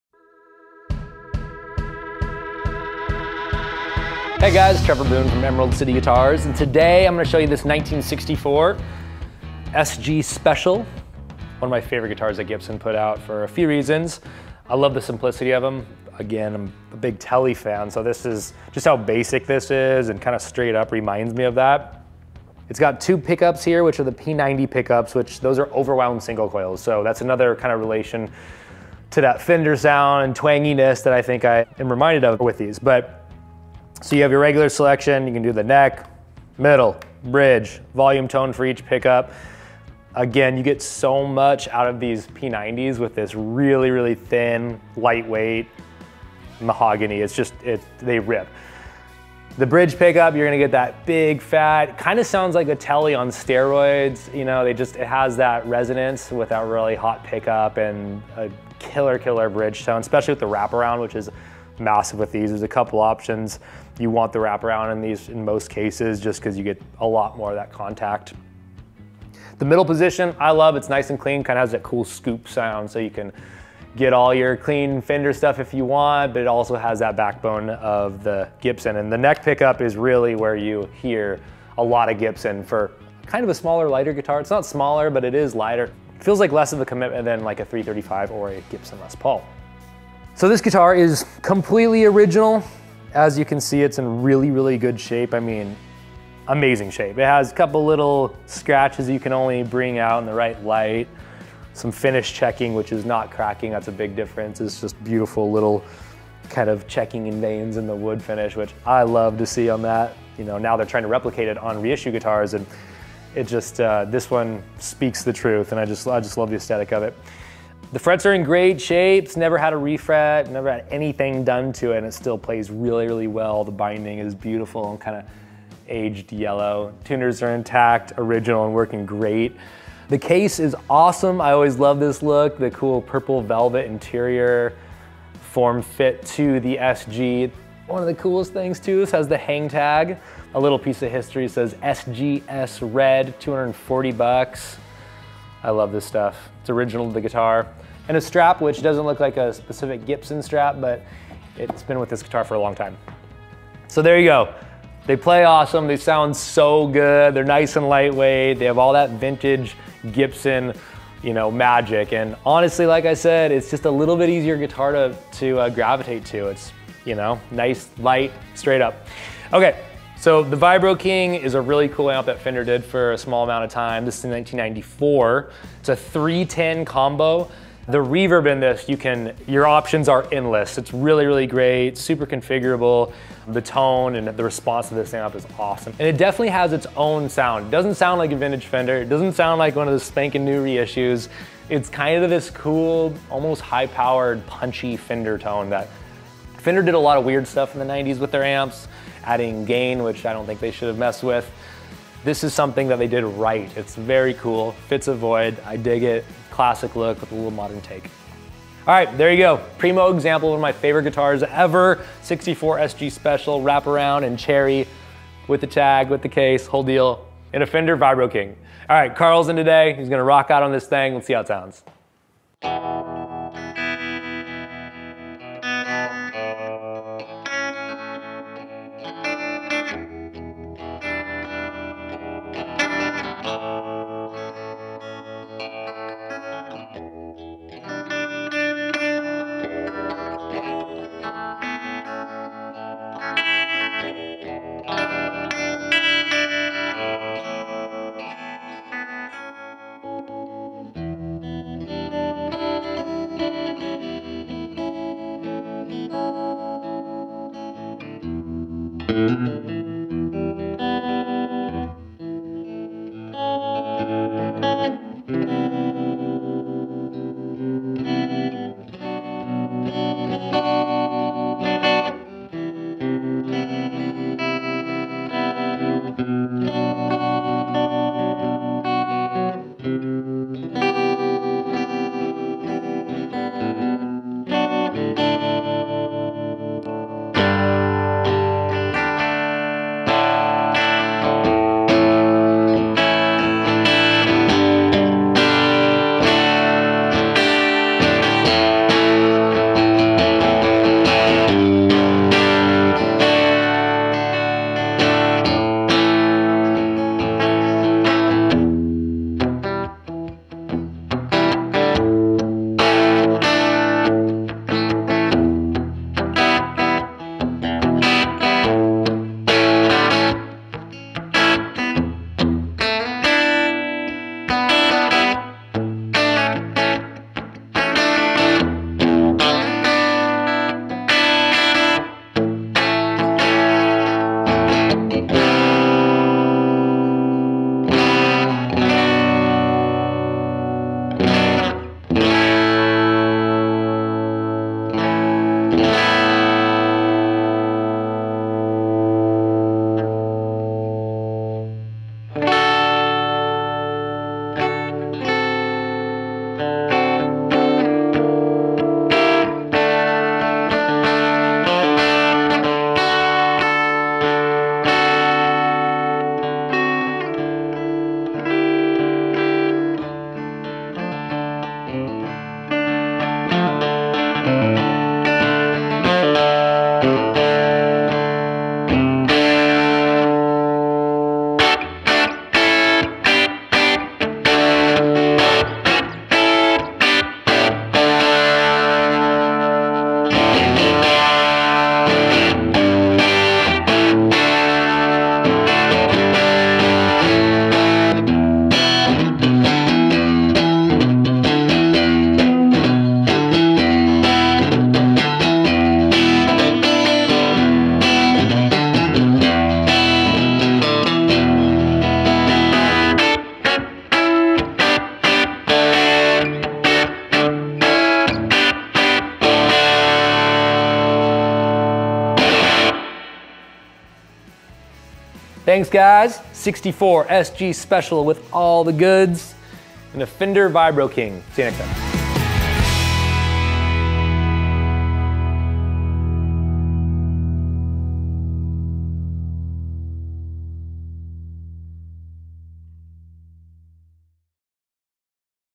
"Pick of the Day" - 1964 Gibson SG and 1994 Fender Custom Shop Vibro-King
Vintage SG day?! It sure is! Today we have a beautiful 1964 Gibson SG Special, loaded with killer P90s and of course in the deep red finish in great condition. This guitar is original from top to bottom, including the frets, and plays like a dream! Today we've got Trevor back in the shop to tell you all about it before handing the guitar to Karl for a demo through a lovely 1994 Fender Custom Shop Vibro-King amp, which is a unique 3x10 combo with fantastic reverb and vibrato, with a unique yet still very Fender sound!

For more information on the guitar or amp, please give us a call or check out: